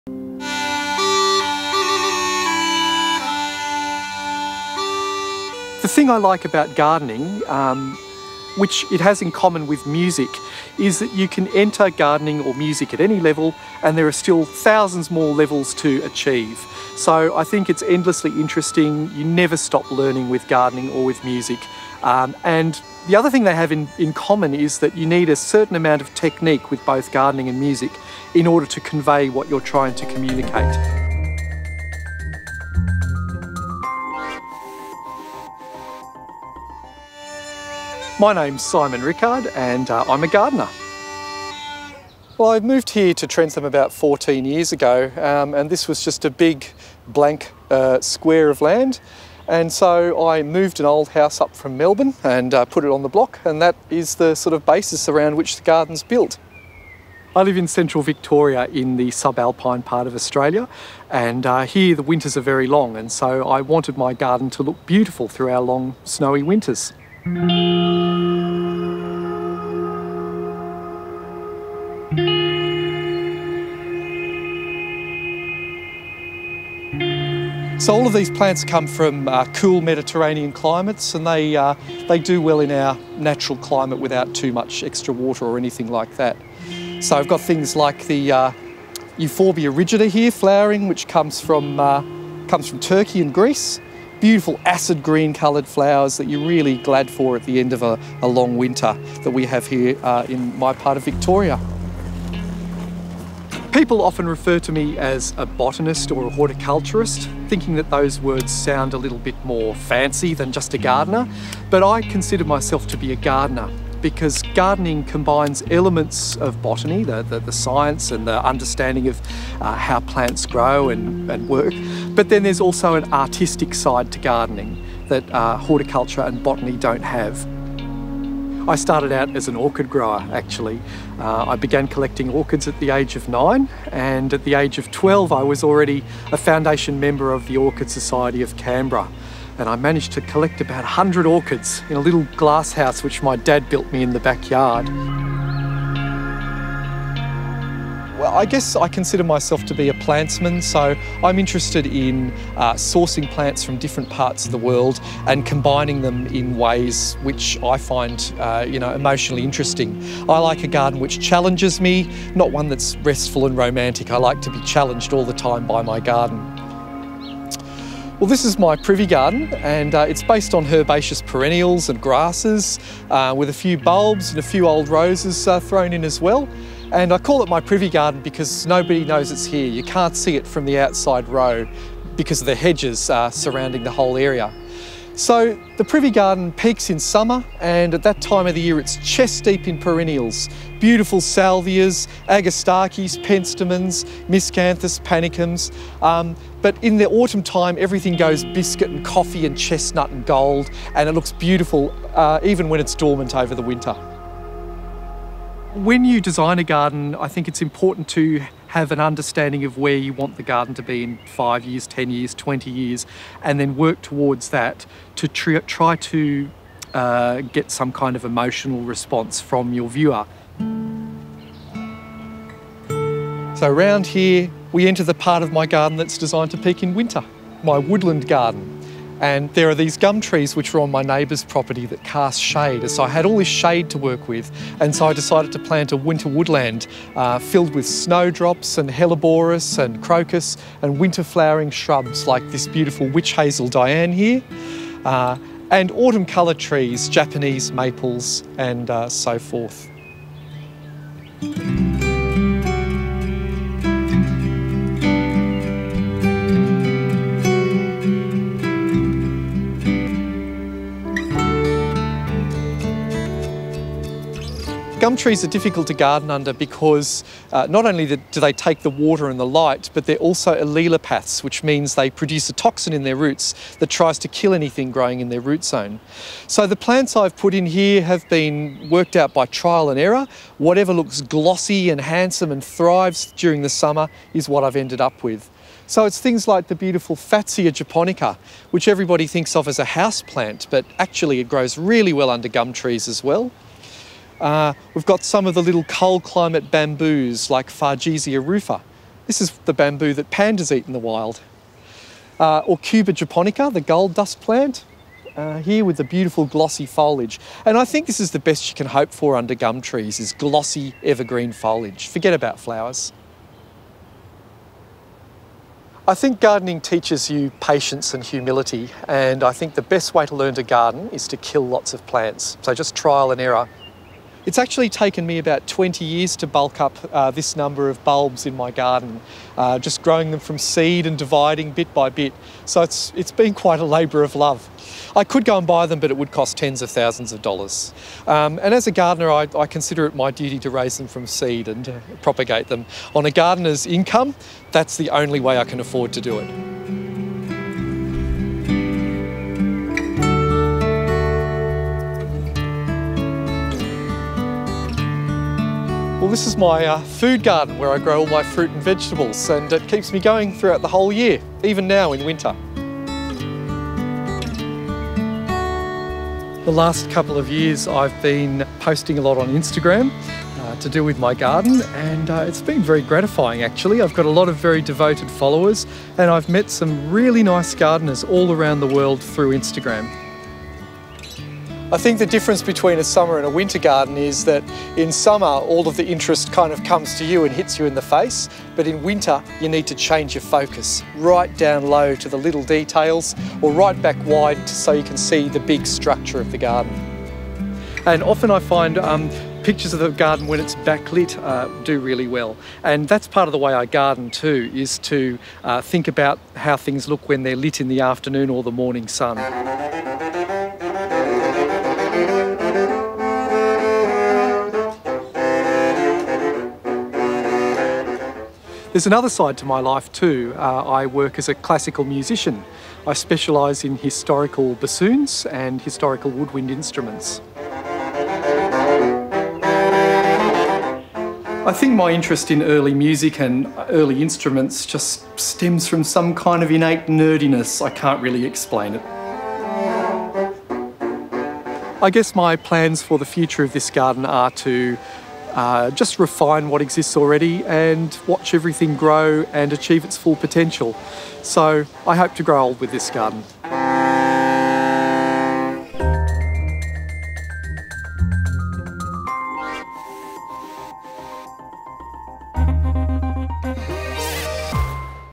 My Garden Path: Simon Rickard
In the cool climate, central highlands of Victoria lies the sleepy hamlet of Trentham. Nestled on the edge of the Macedon ranges and the ever-popular Spa Country of Daylesford and surrounds, Trentham seamlessly blends history and rural character with artistic, creative, diverse and, at times, downright quirky undertakings of the town and its’ residents. If there is one place that encapsulates the feel of this special township, it is the private garden of Simon Rickard – a man with as many facets as the town he now calls home.

Simon has always been a gardener and alongside his grandmother started an orchid collection around age nine. This collection grew and, by the age of 12., Simon was the founding member of the Orchid Society of Canberra. Having spent time working within gardening and horticultural pursuits, as well as time as a musician, Simons passion for gardening is obvious as you enter his property. There is an artist’s eye at play in this space, which was nothing more than a blank canvas when Simon moved himself (and a small house) there from Melbourne 12 years earlier.  “We are in the sub-alpine area of Victoria here” says Simon “so the garden design truly had to incorporate plants that would cope with our extended winters, and look beautiful”.

Plant selection is key to this gardens winter success, with many of the plants in his garden hailing from cool Mediterranean climates, with seasons like those experienced in Trentham. Gopher Spurge (Euphorbia rigida) from Turkey and Greece is one such plant, its acid green foliage contrasting spectacularly against vibrant yellow flowers with splashes of bronze. “I consider myself a plantsman, I’m interested in sourcing plants from different parts of the world and combining them in ways that I find emotionally interesting. I like a garden that challenges me, not one that is restful and romantic” explains Simon.